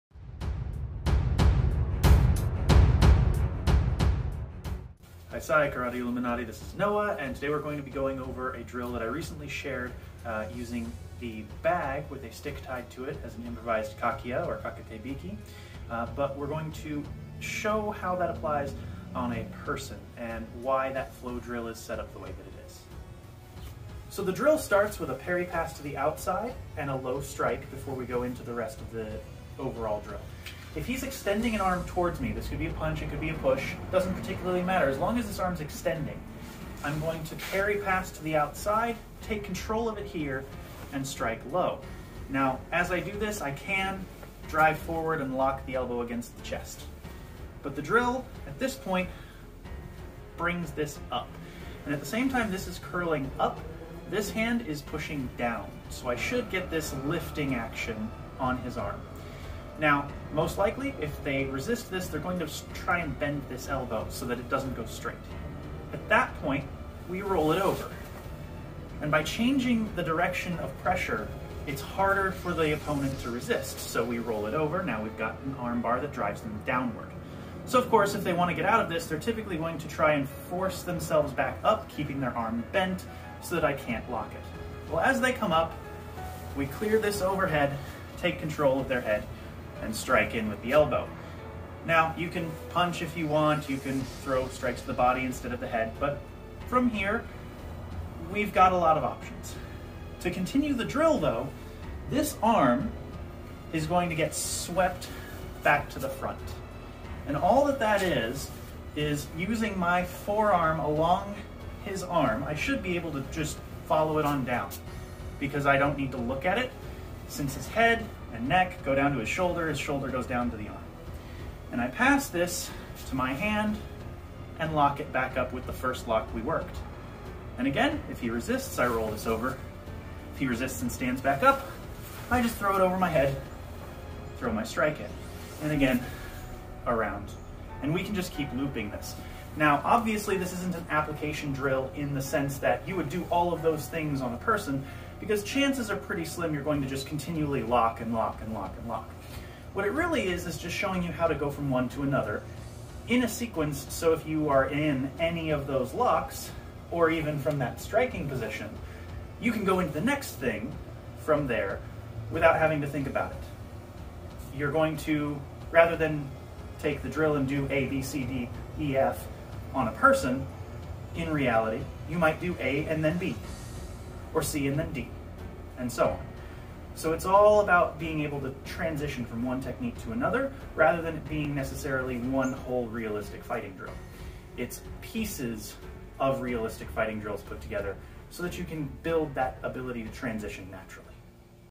Muchimidi Lock Flow Drill
A breakdown of a drill I recently demonstrated on social media using an improvised kakiya/kakete-biki